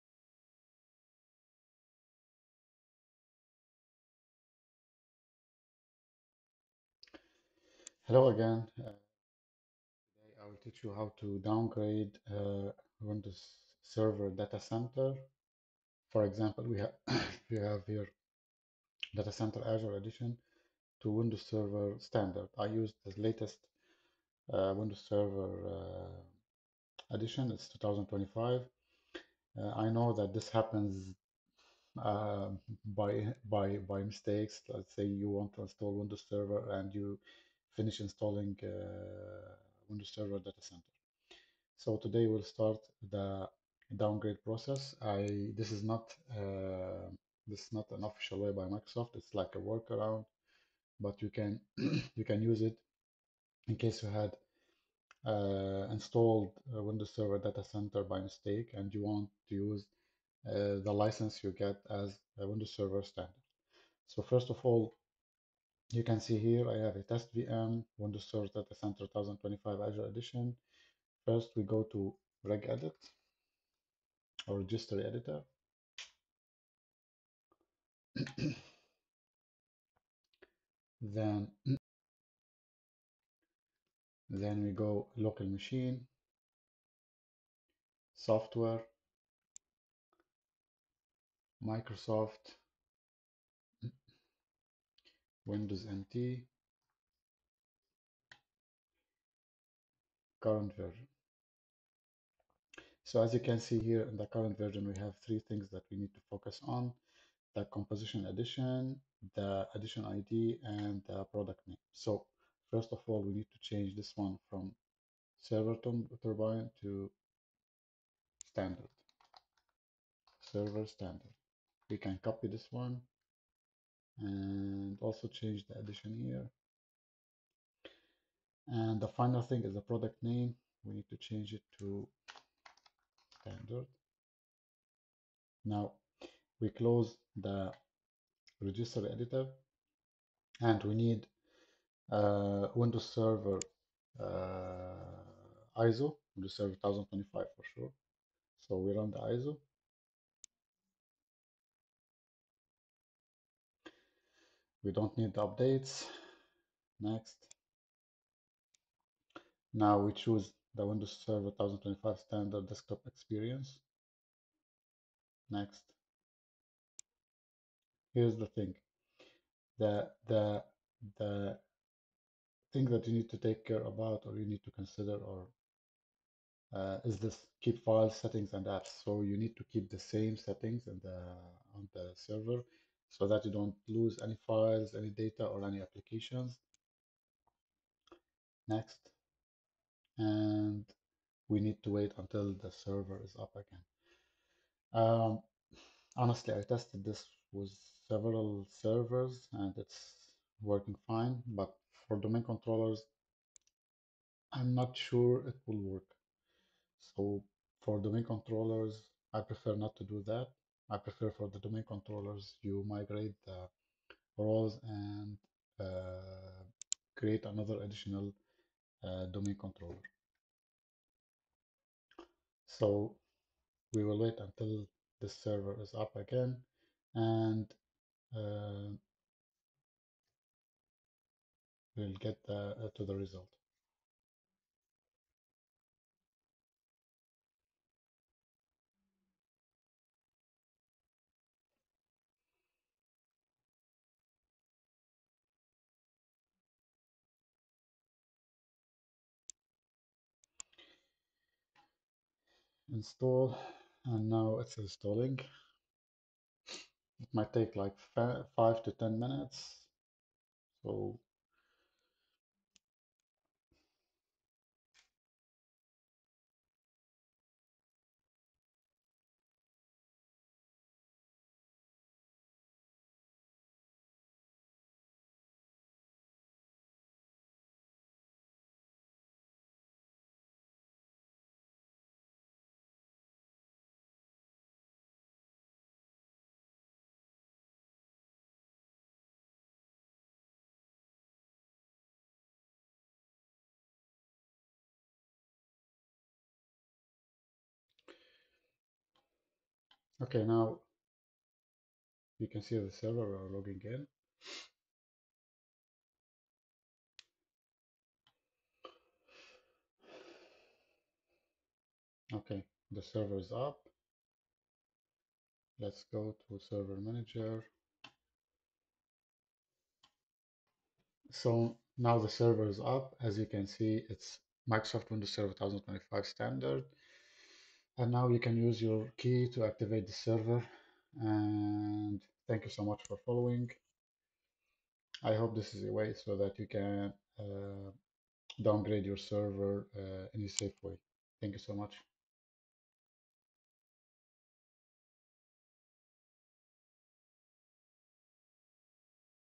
Downgrading Windows Server DataCenter to Standard Edition
Microsoft does not officially support downgrading Windows Server DataCenter to Standard Edition as a direct method. However, a workaround involves modifying the registry and running the official Standard Edition ISO. This method can facilitate a stable transition while preserving system functionality.

Before proceeding, it is highly recommended to back up all critical data and configurations to prevent any potential data loss. Do not apply this method on domain controllers (DCs) unless you have a verified backup.

Registry Modifications:
Navigate to:
HKEY_LOCAL_MACHINE\SOFTWARE\Microsoft\Windows NT\CurrentVersion

Modify the following values:

EditionID = ServerStandard
CompositionEditionID = ServerStandard
ProductName = Windows Server 20XX Standard (Replace with 2019, 2022, or 2025 based on your version)

Also you can downgrade from Standard to Essentials :
EditionID = ServerSolution
CompositionEditionID = ServerSolution
ProductName = Windows Server 20XX Essentials

After making these changes close regedit and proceed with running the Standard Edition ISO to complete the downgrade process from DC to Standard and run the Essentials iso to complete the downgrade from Standard to Essentials.